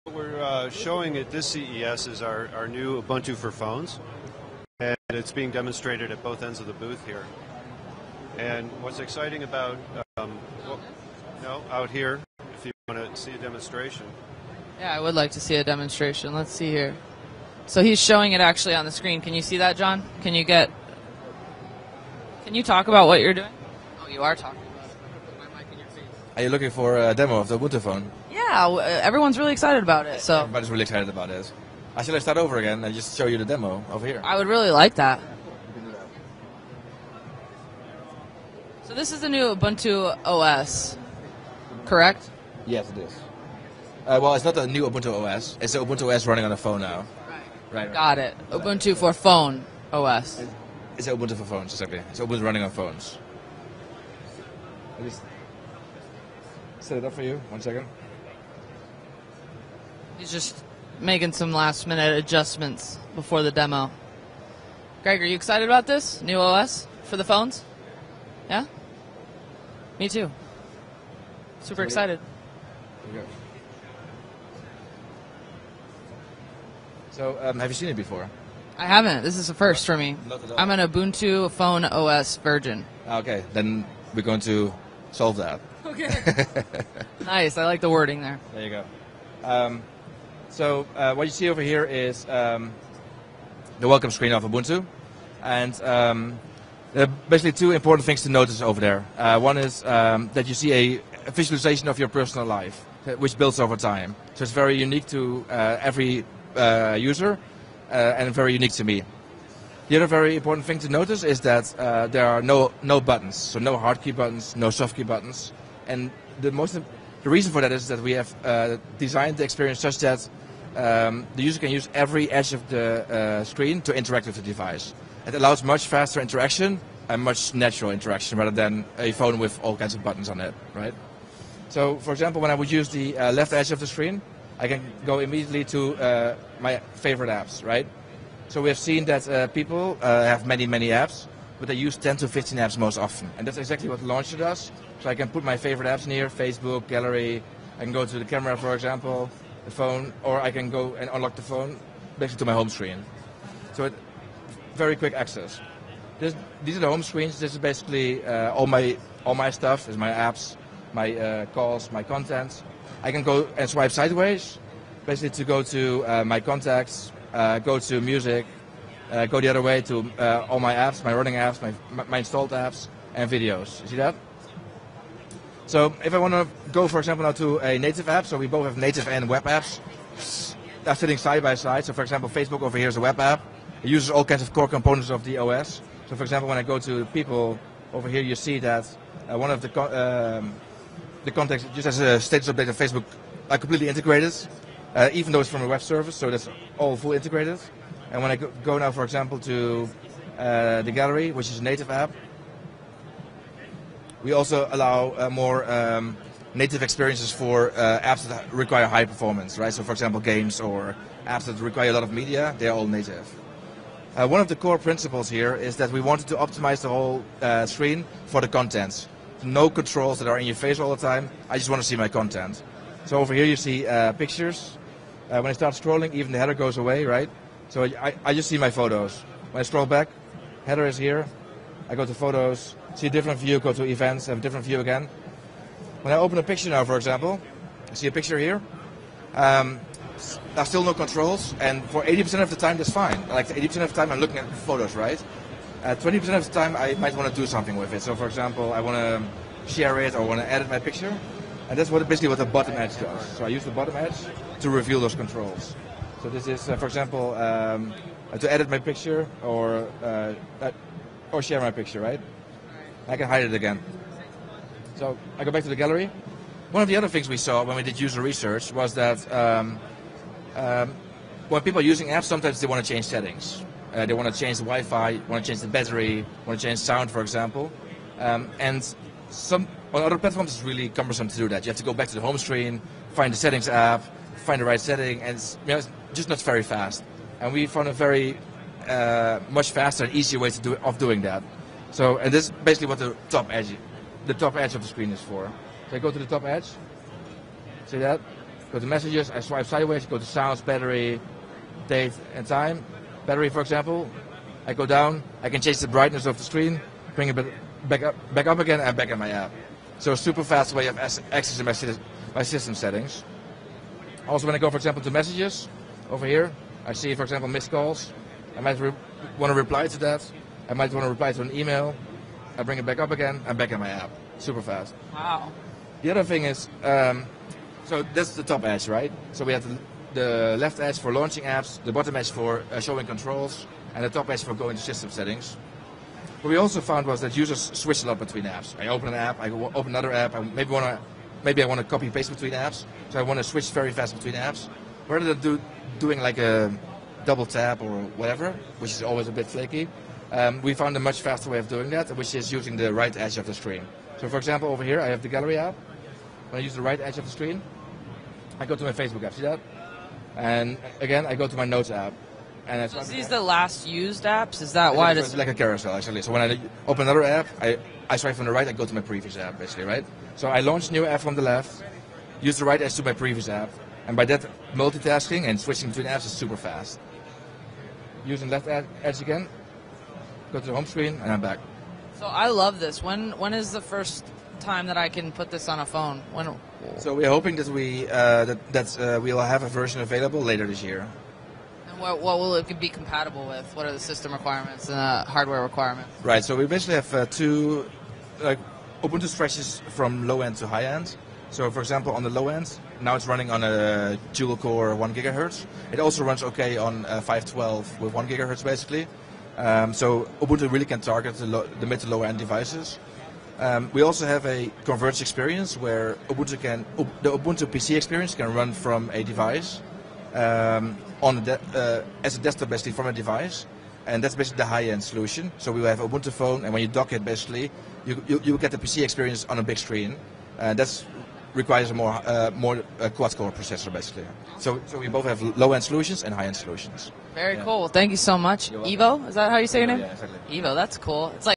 Ubuntu Phone OS | TechCrunch At CES 2013
Ubuntu's mobile OS was one of the most interesting recent announcements in mobile tech, and at CES 2013 we finally got a chance to go hands-on and receive a thorough demo of the OS in its admittedly early current stage. Despite the fact that even a developer launch of Canonical's Ubuntu Mobile is still likely a few months off, it's already looking like a strong contender, and potentially one that could shake up a market that has had relatively few big earth-quaking changes over the past couple of years.